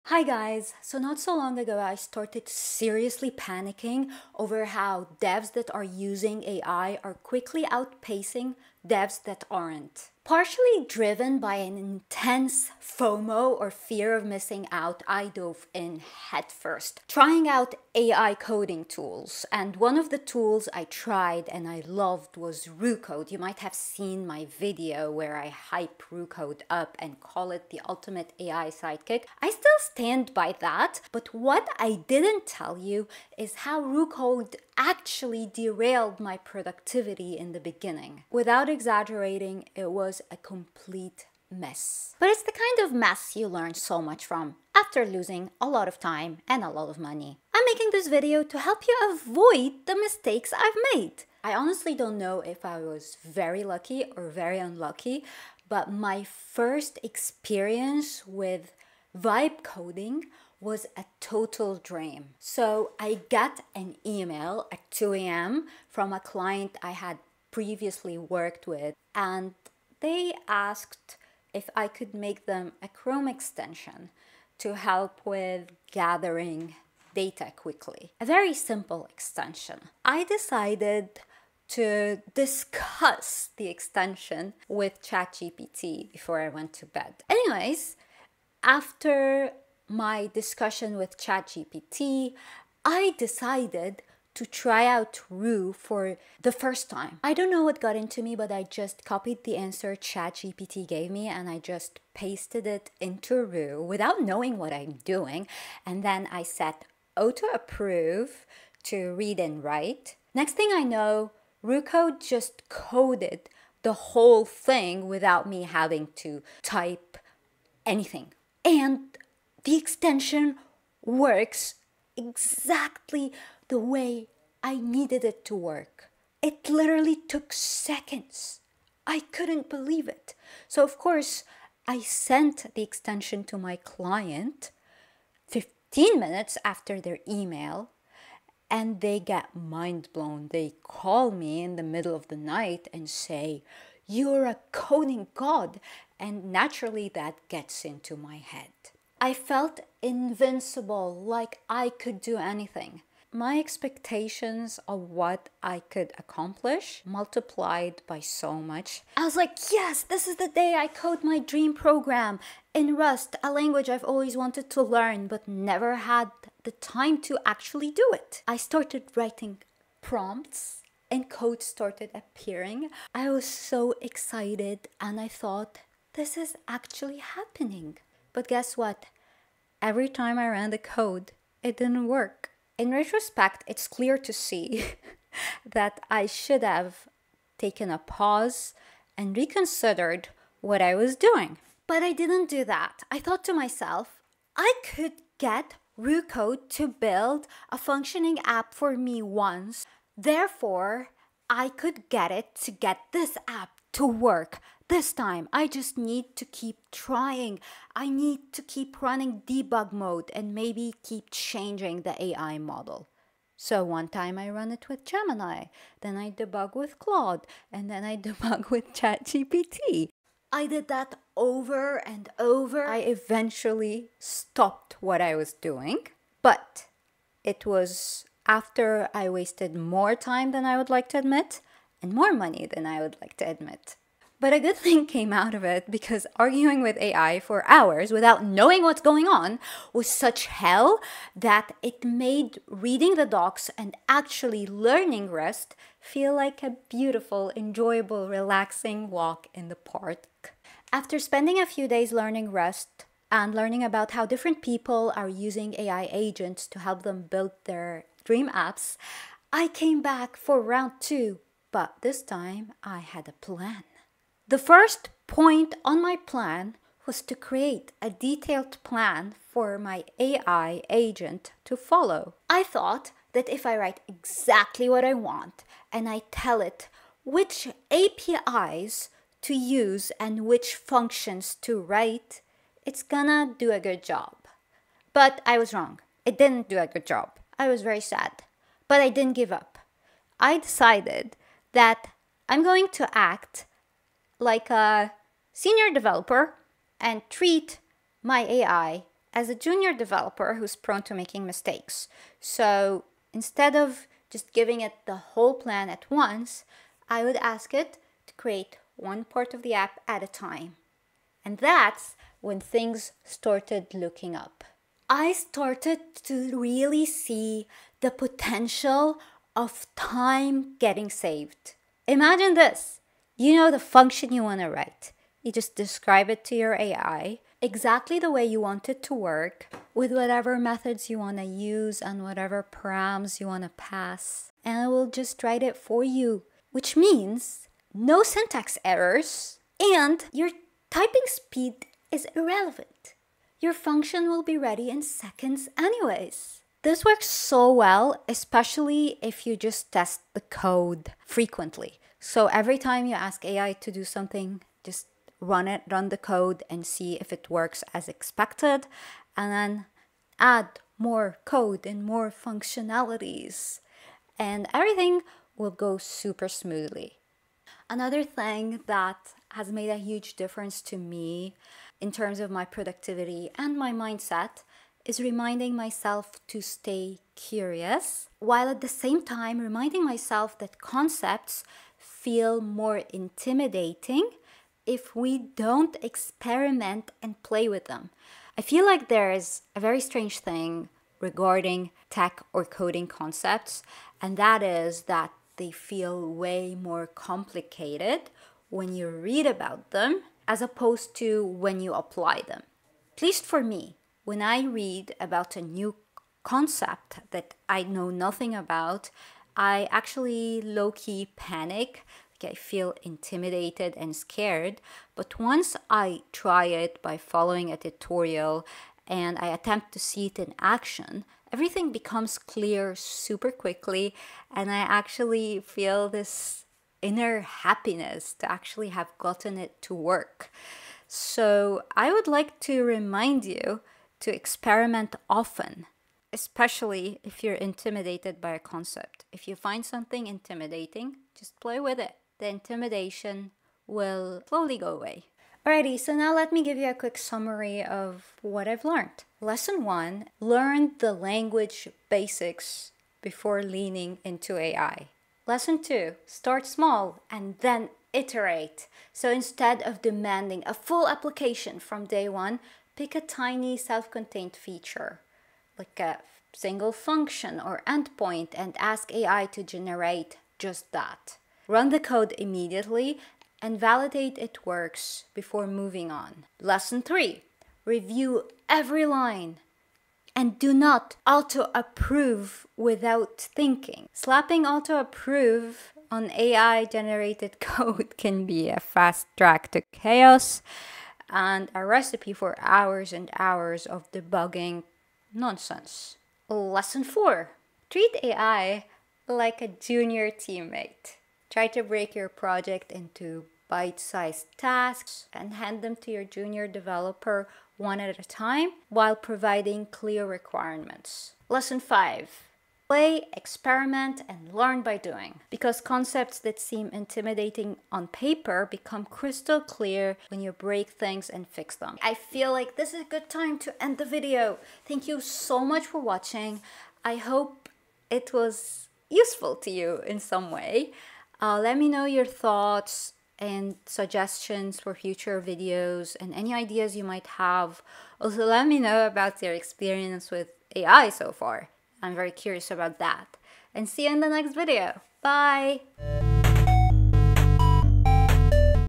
4 Things I Wish I Knew Before Coding with AI
In this video, I share my real experience using RooCode, an AI coding assistant, and how it almost wrecked my workflow. I dive into common AI coding mistakes, the challenges of using AI tools for programming, and how I overcame burnout by learning Rust the right way. Whether you’re new to AI developer tools or looking to improve your coding productivity, this video has valuable lessons for you.

Not long ago, I had a major FOMO meltdown watching developers using AI tools to level up faster than ever. So I dove into the world of AI coding and things got wild. In this video, I share my honest experience using Roo Code, from building a Chrome extension in literal seconds (felt like magic) to completely wrecking my productivity with overly ambitious goals (hello, Rust).

What we'll discuss:

- How I accidentally auto-approved AI code that cost me time and money

- Why my first success gave me false confidence

- The big mistake of relying on AI without learning the basics first

- How treating AI like a junior developer changed everything

- Real strategies to stay productive, curious, and in control while using AI

If you're just getting into AI-assisted coding—or you're wondering why things feel so chaotic sometimes; this video is for you. I break down the painful lessons and the huge mindset shift that finally helped me unlock AI's real power (without losing my sanity).

⏲️ Timestamps & Summary

00:00  Panic & AI FOMO
00:30  Diving into AI Coding Tools: My First Steps with Roo Code
01:15  Instant Win: Building a Chrome Extension with ChatGPT + Roo Code
02:00  Post-Success Hype: Aiming for a Rust App with AI Assistance
02:30   Why my AI-Generated Code Kept Failing
03:15  Debugging Chaos: Switching Between Gemini, Claude & ChatGPT
04:00  Turning Point: Learning Rust Fundamentals After Wasting Time
04:45  Second Attempt: Crafting a Detailed Plan for AI Agents
05:30  Plan Failure: Why Even Detailed AI Prompts Can Fall Short
06:00  Treat AI As a Junior Teammate – Breaking Tasks Down
06:45  Iterative Development: Running & Testing AI-Generated Code Frequently
07:20  Embrace Experimentation – Learning by Doing
08:00  Key Takeaways: 5 Essential AI Coding Lessons